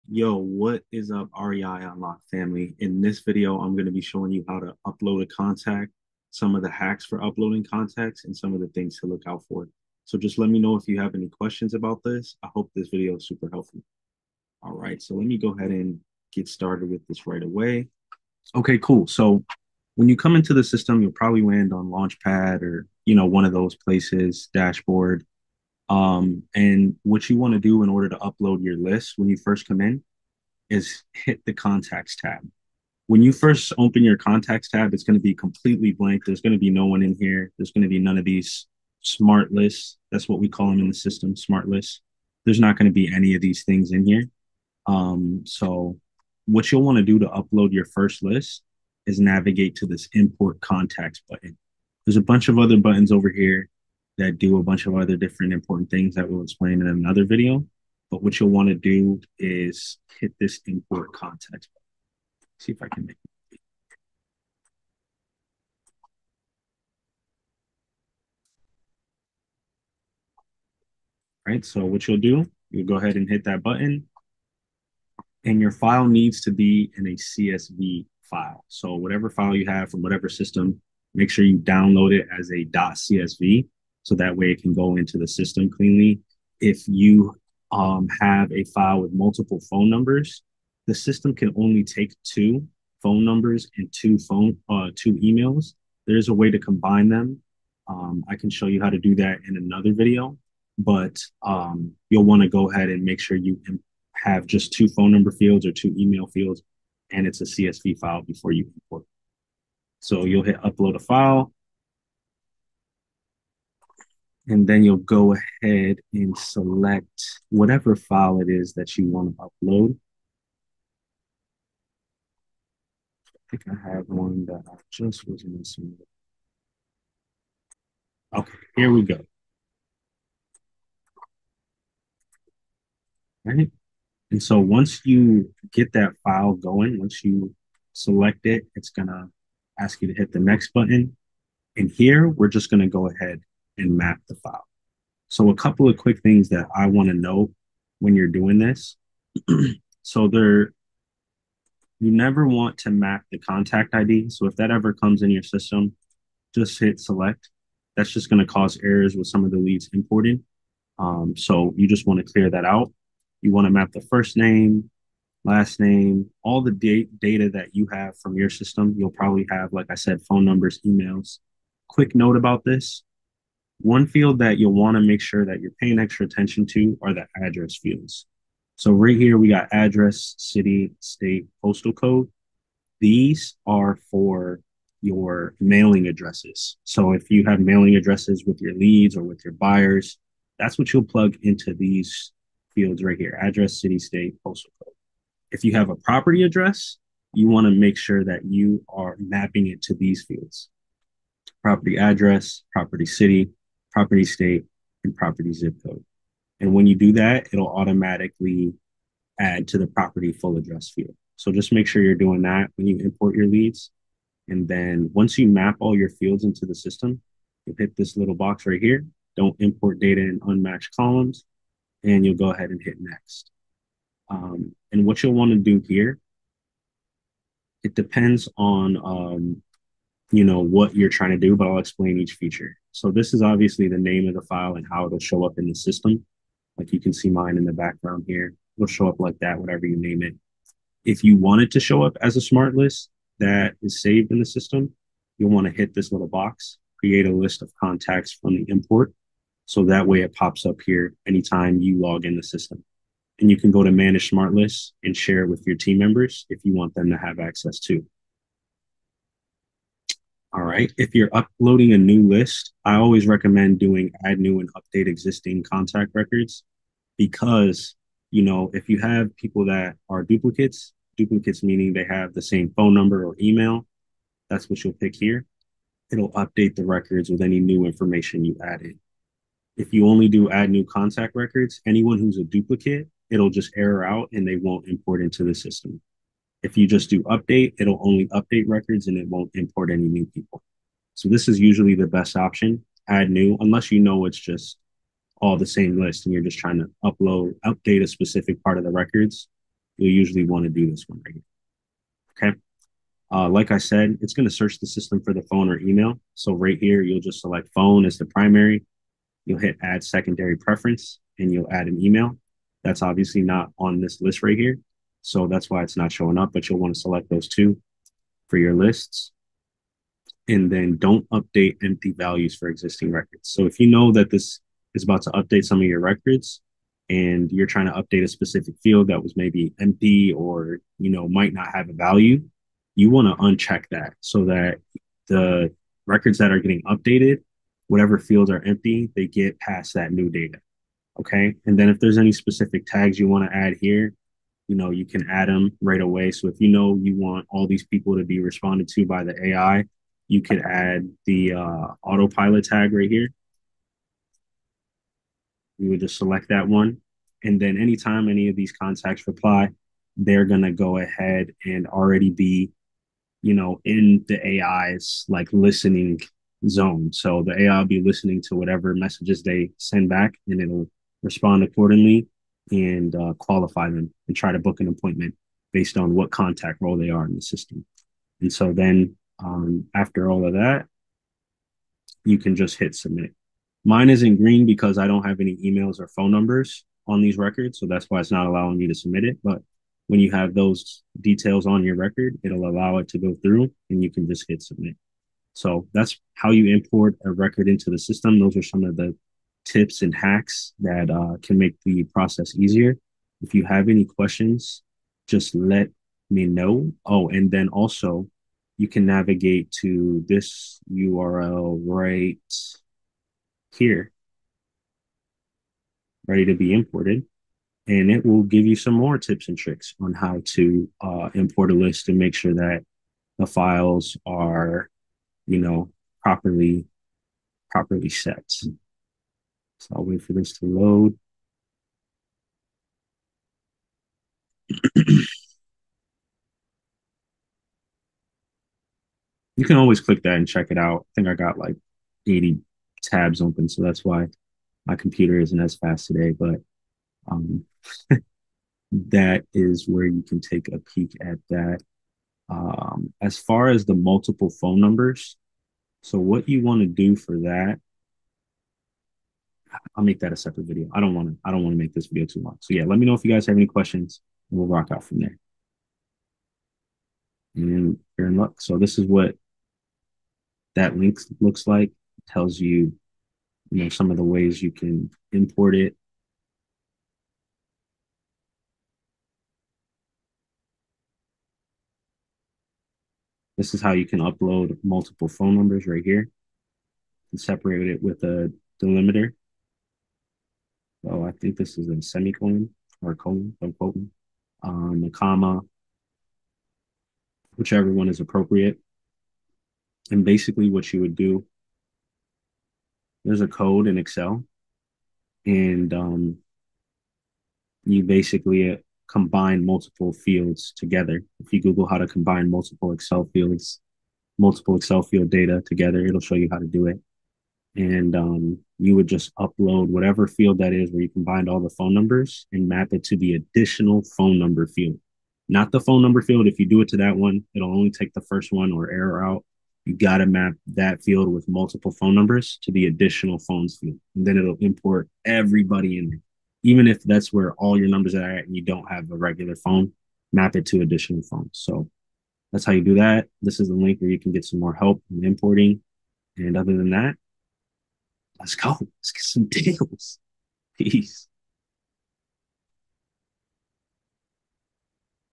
Uploading Contacts to REI Unlock
This video talks about one of the most important steps needed after signing up - Uploading Contacts to REI Unlock

Let's get some leads and deals rolling!!!